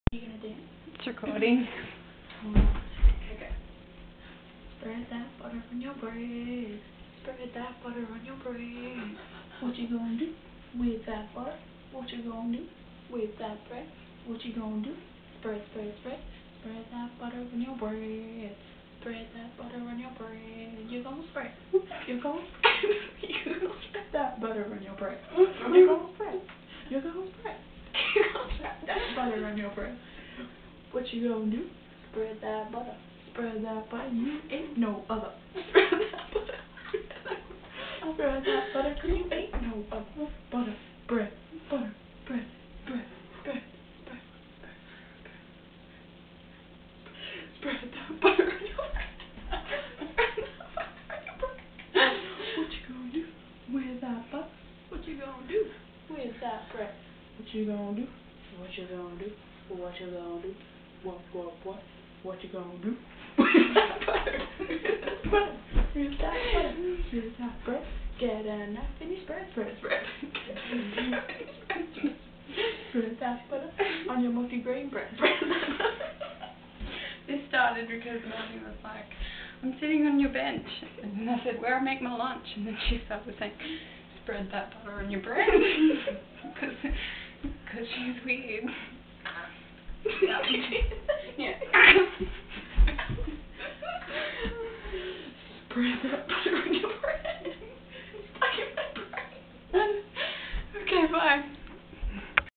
Spread that butter
me and marnie spreadin that butter on that bread.